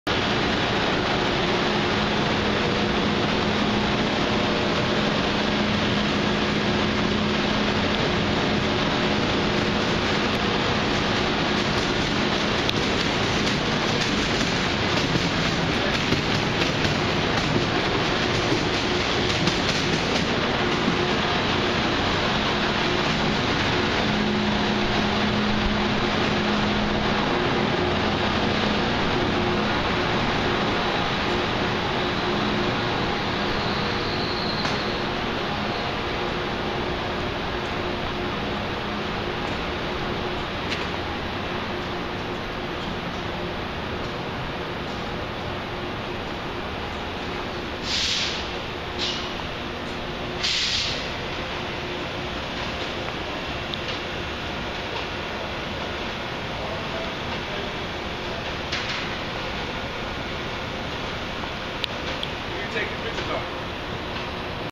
Amtrak Trains At South Station
2 Acela Express trains on the same platform side by side at South Station, plus a regular amtrak train with amfleet coaches on another platform. If your wondering why that guy in the video was asking what I was taking pictures of, do know that your permited to take pictures at south station, and when I told him I was photographing the trains, he didn't mind.